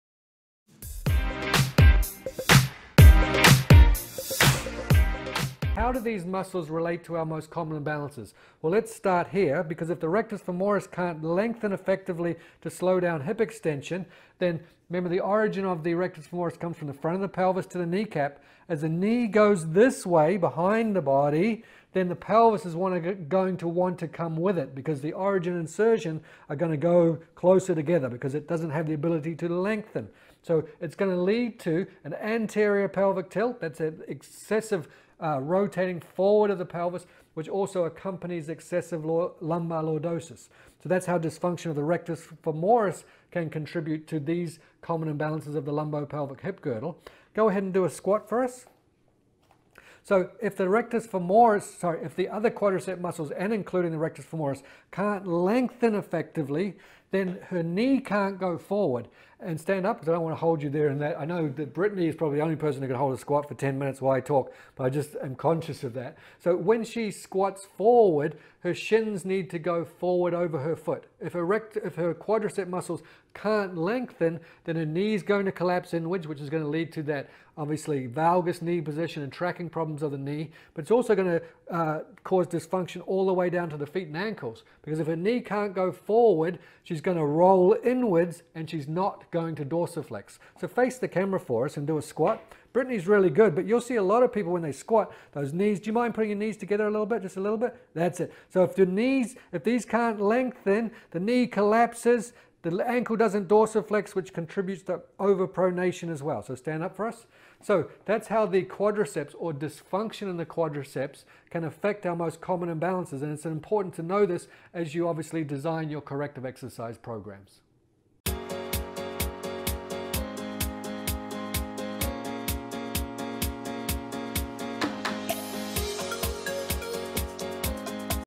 Quadriceps Muscles: Their Role in Lower Back Pain, Ankle Pain and Knee Pain
Watch this video to learn how the quadriceps muscles can cause lower back pain, knee pain and ankle pain when they are not working correctly from The BioMechanics Method Corrective Exercise Specialist Certification.